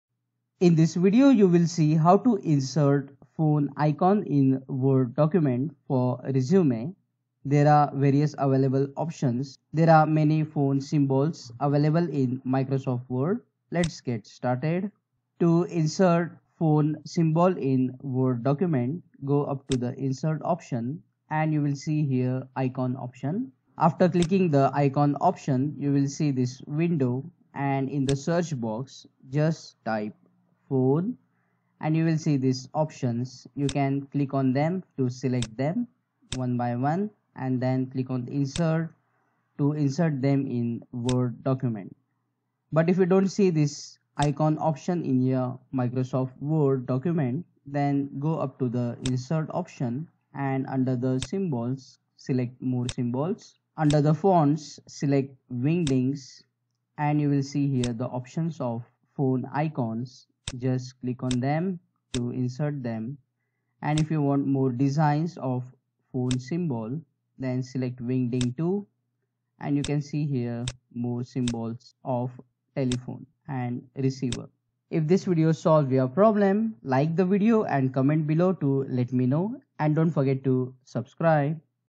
How to Insert Phone Icon in Word (Microsoft)
Watch in this video How to Insert Phone Icon in Word (Microsoft) document for resume. Use the icon option in Ms word or Insert symbol option to insert phone symbol / Logo in word document